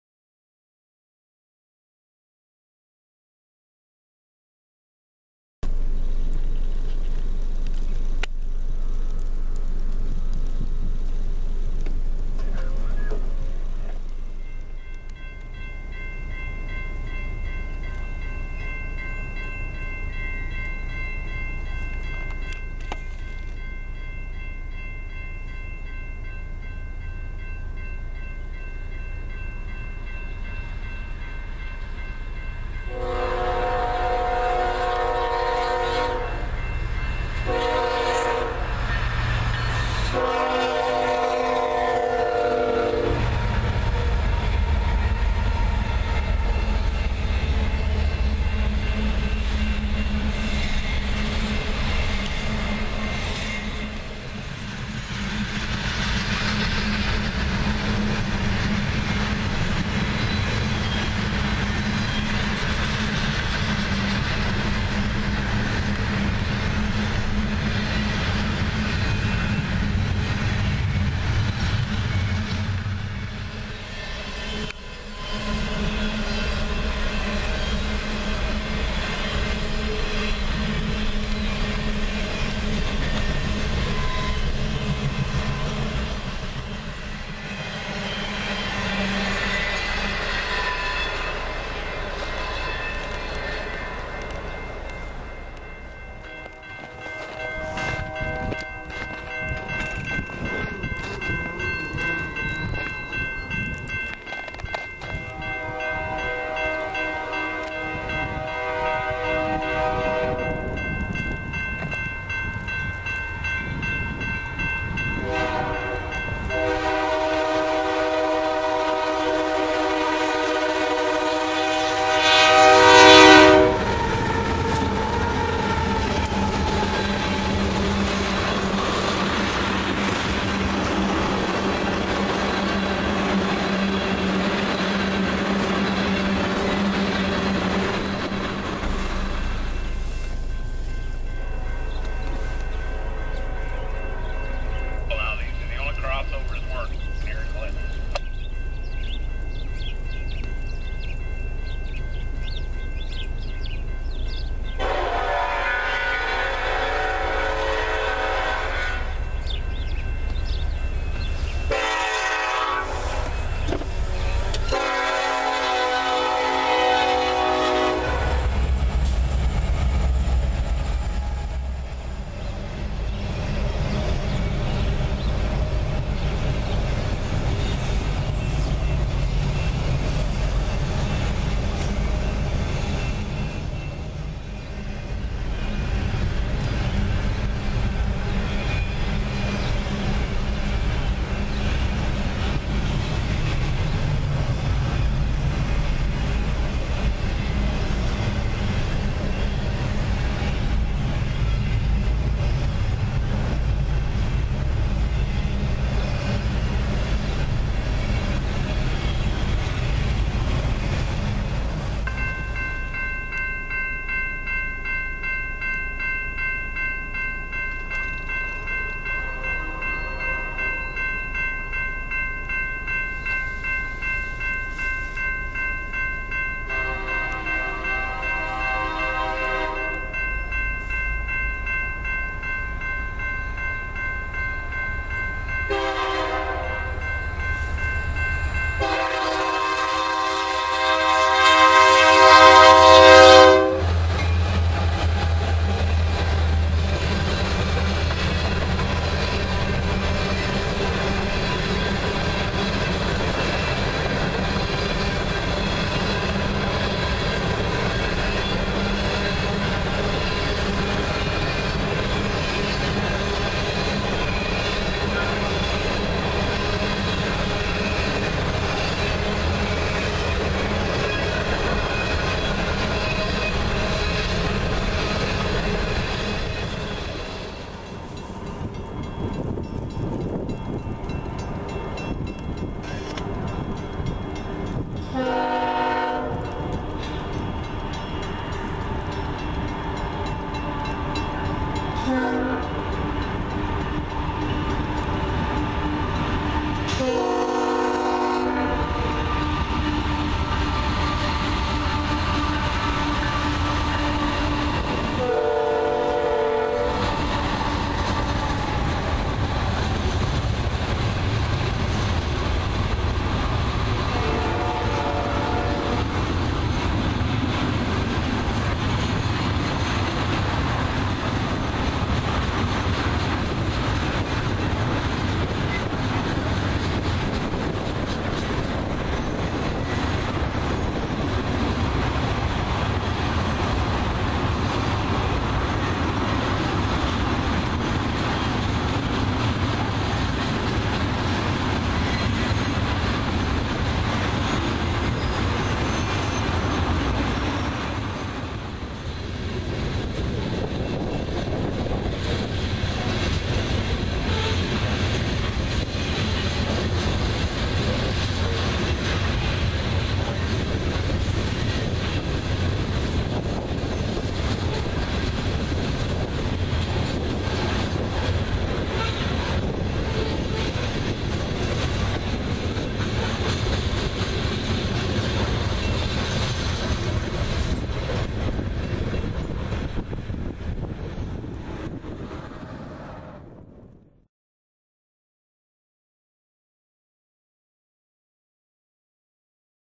New Years Day by the Rails
Busy action along the Union Pacific Clinton Sub and the Iowa Interstate Cedar Rapids Sub. January 1, 2014.